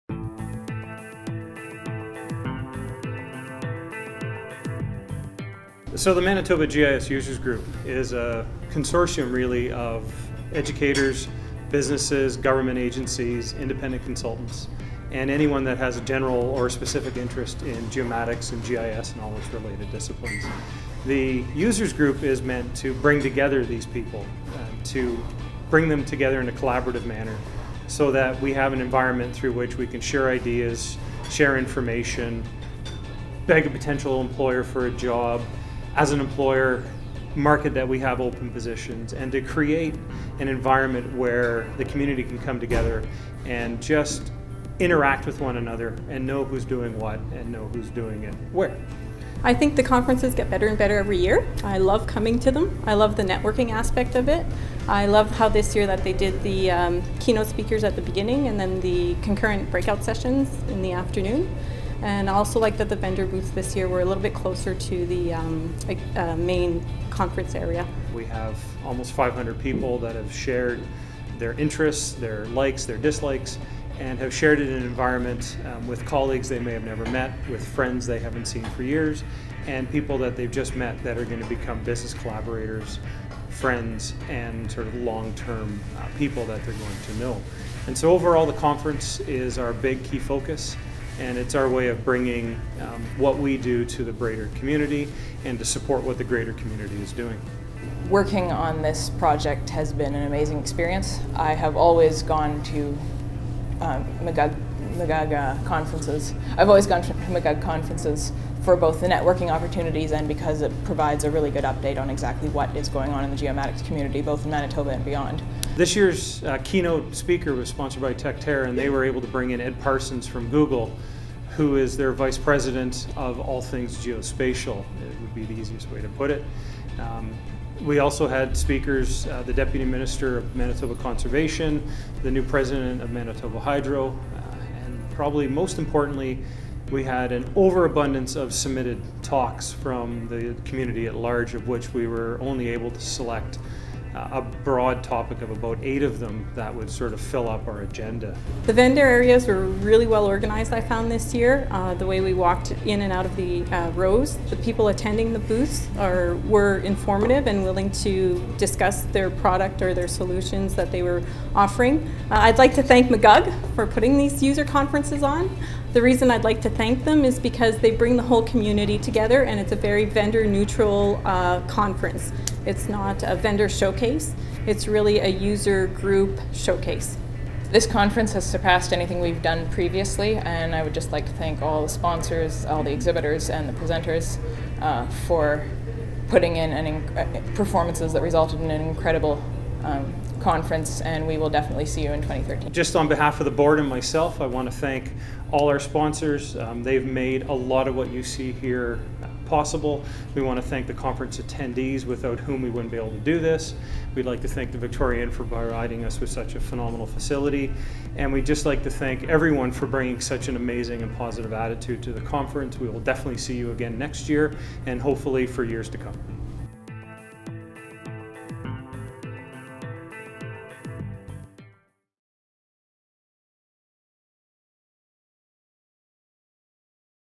MGUG Fall Conference 2012
The Manitoba GIS User Group (MGUG) Fall Conference is Manitoba's premier geospatial information conference.  The 2012 event attracted over 400 attendees from industry, government and academia and included a vibrant exhibitors area.  Additional information on the 2012 Conference and MGUG is available